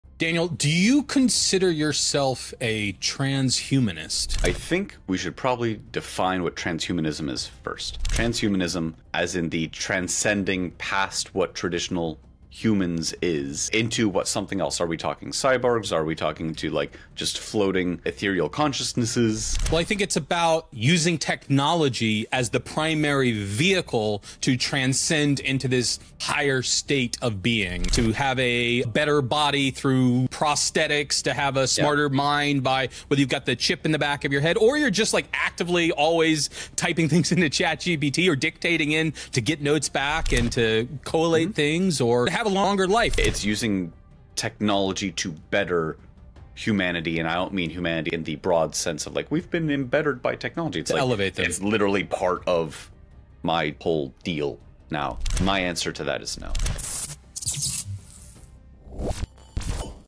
Transhumanism? You Might Be Living It 👁️‍🗨️🤖
Transhumanism isn’t just sci-fi anymore. It’s not just cyborgs or floating minds—it’s using tech to transcend what it means to be human. From prosthetics to ChatGPT-enhanced creativity, we’re already stepping beyond biology. The question isn’t if transhumanism is real—it’s how far you’re willing to go with it.